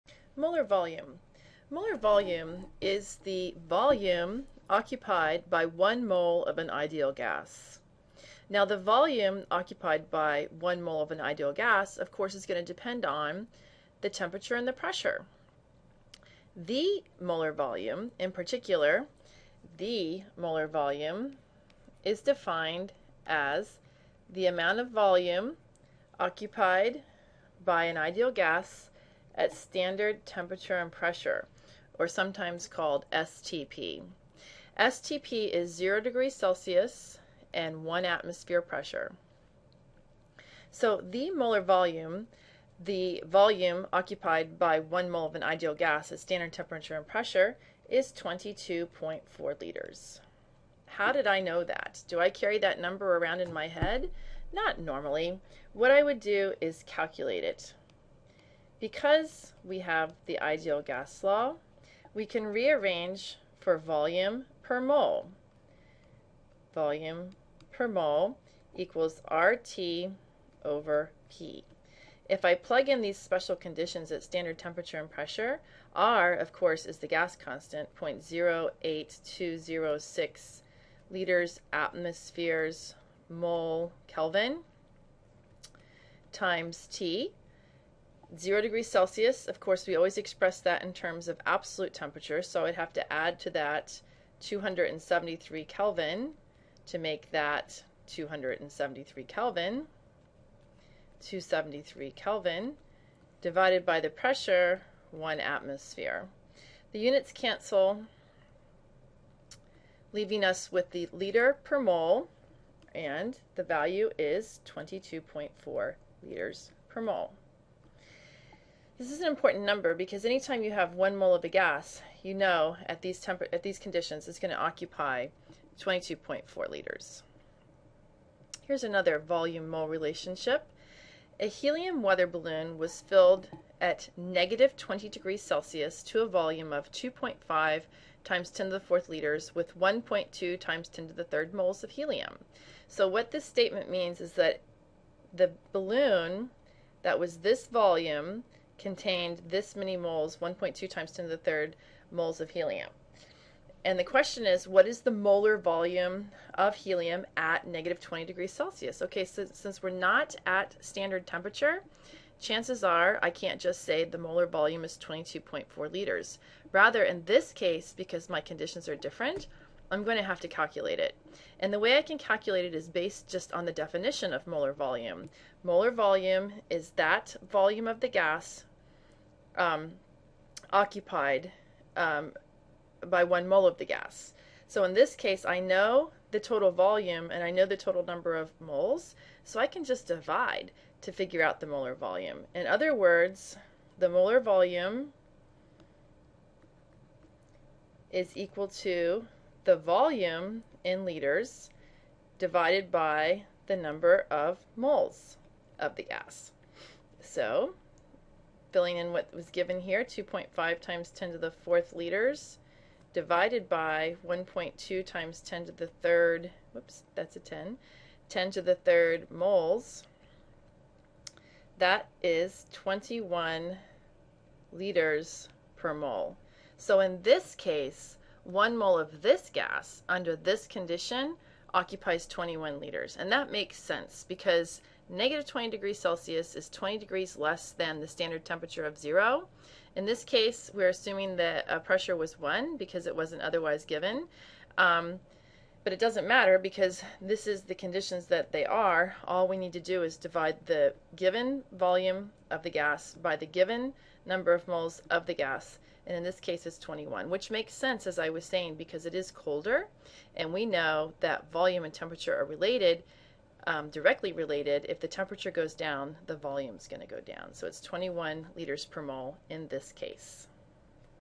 Molar Volume Example
This short video defines molar volume and shows by example how one could determine the molar volume of a gas at standard and nonstandard conditions.